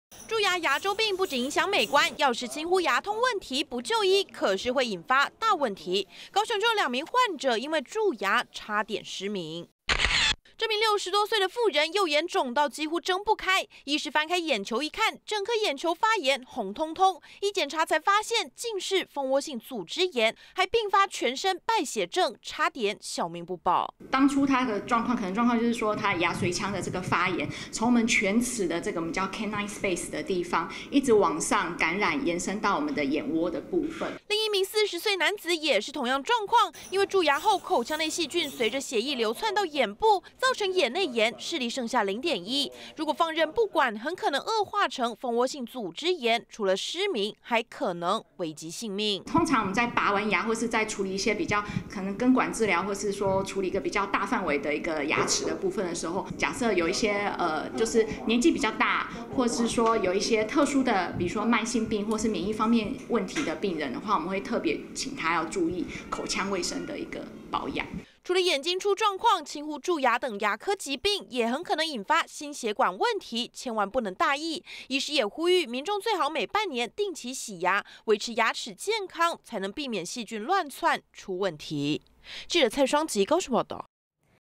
20170904中天新聞 眼睛紅腫痛險失明 竟是蛀牙細菌惹禍！
眼睛紅腫痛，視力出問題，竟然是蛀牙惹禍！高雄兩名蛀牙患者，因為輕忽牙痛問題，導致口腔內細菌隨著血液流竄到眼部，引發眼內炎和蜂窩性組織炎，視力大受影響差點失明，要是放任不管，可是會危及性命。